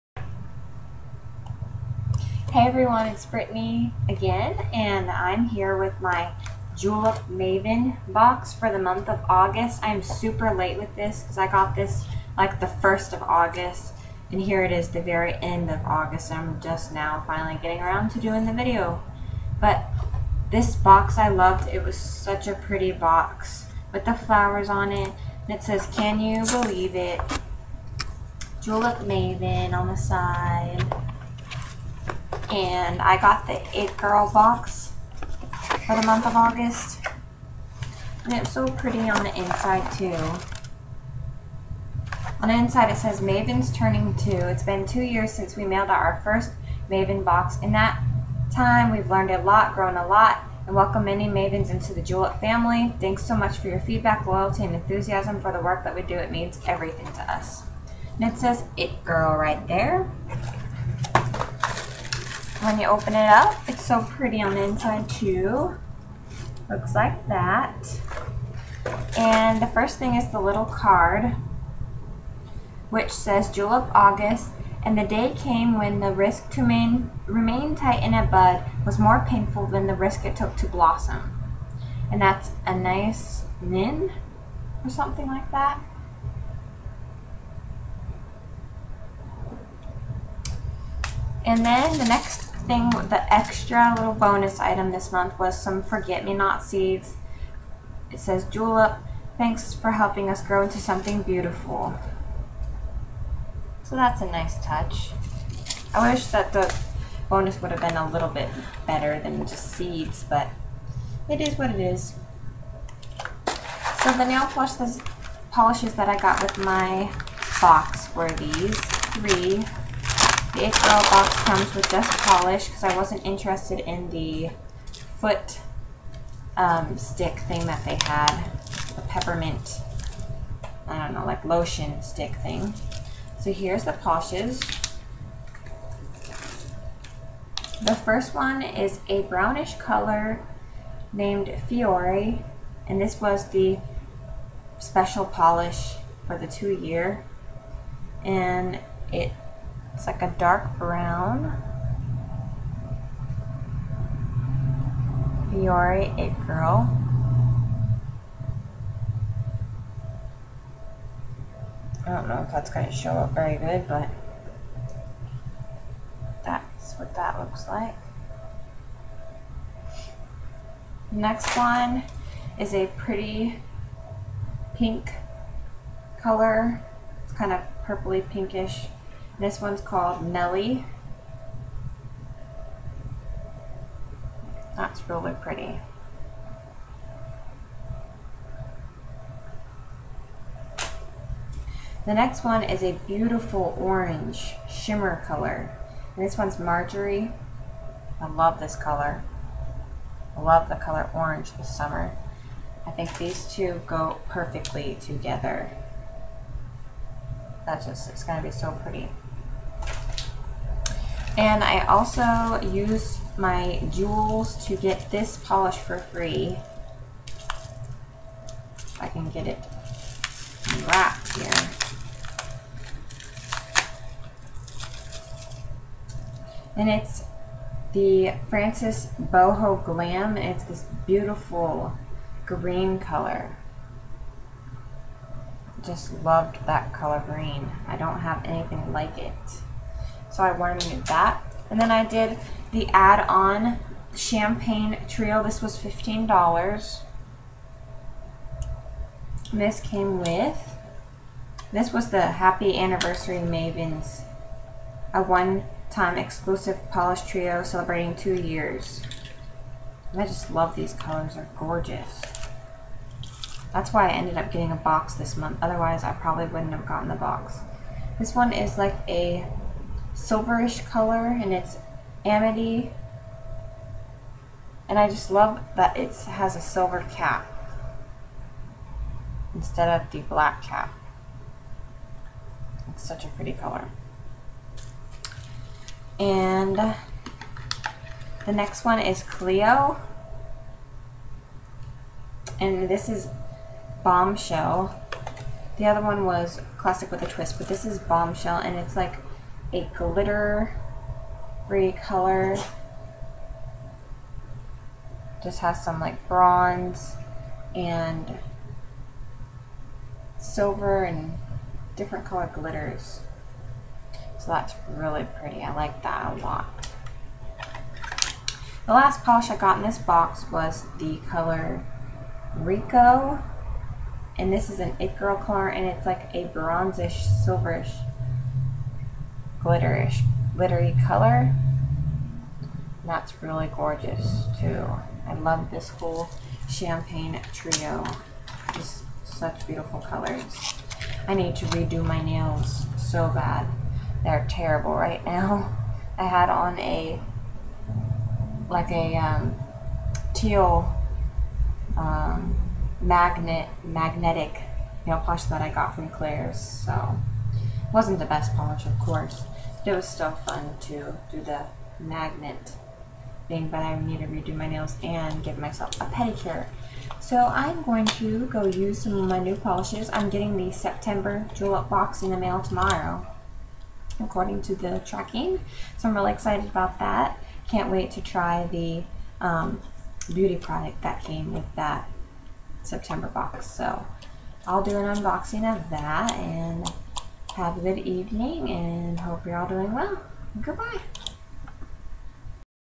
Julep Maven August 2013 unboxing
Unboxing of my Julep Maven box for the month of August.